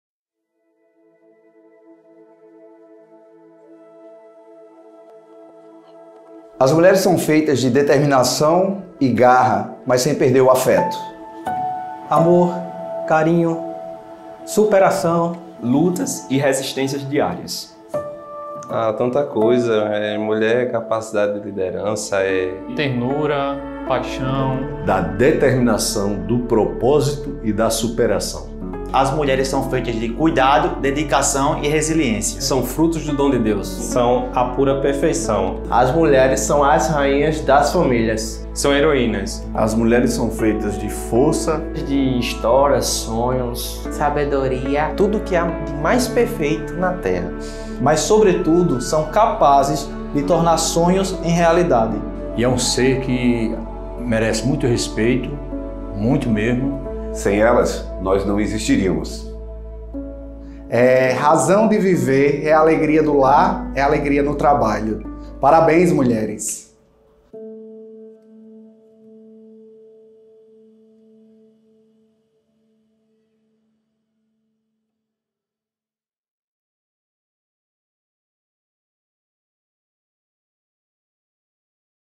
8 de Março | Marcos Inácio Advogados
Vídeo interno (para a intranet do escritório) produzido para a Marcos Inácio Advogados, em João Pessoa - PB.